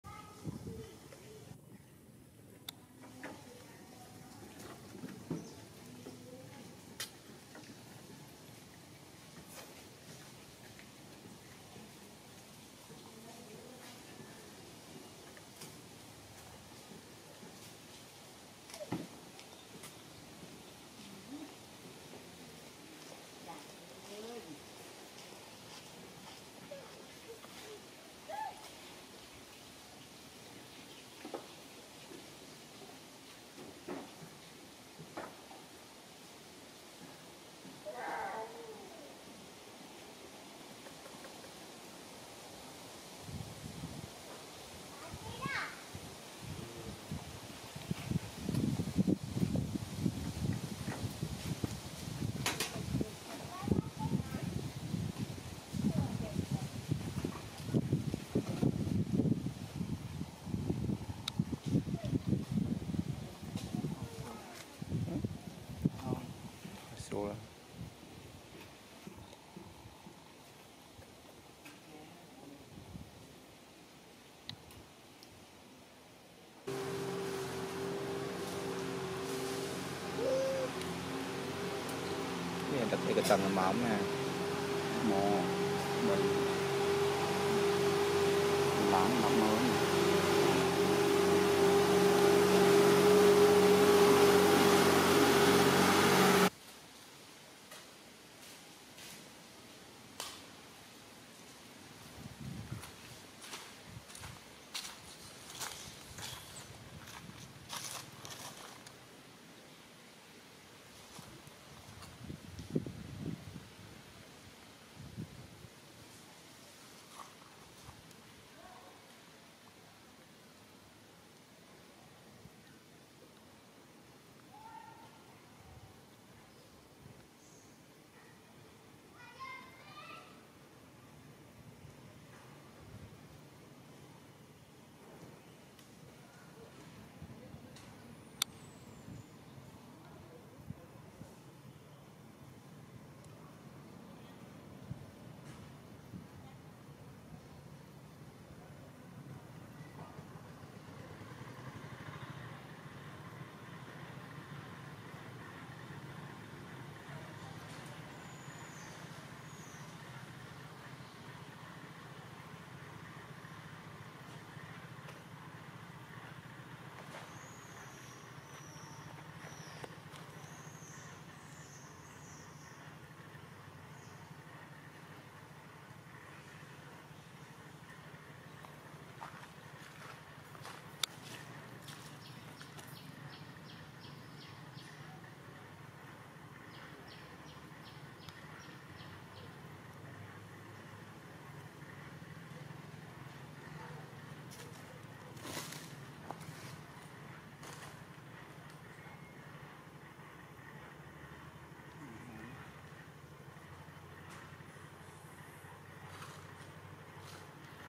Best Mommy....Mother Give Milk In Order Her baby To Rest Peacefully
Welcome To "Love Wildlife TV" Channel
Video Today: Best Mommy. Mother Give Milk In Order Her baby To Rest Peacefully
Best regards